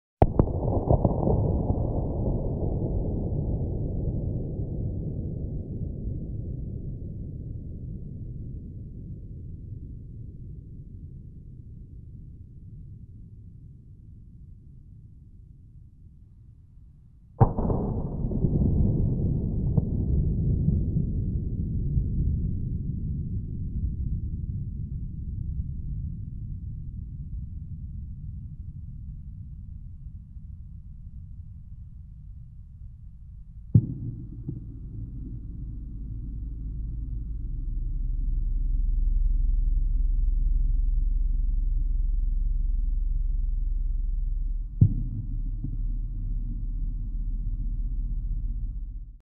SEVER: The Arctic Frontier [teaser]
Strelka project SEVER reconceptualizes the Arctic Circle.
In collaboration with INRUSSIA’s production team, SEVER created an interactive website to help visitors familiarize themselves with the project’s methods and ambitions.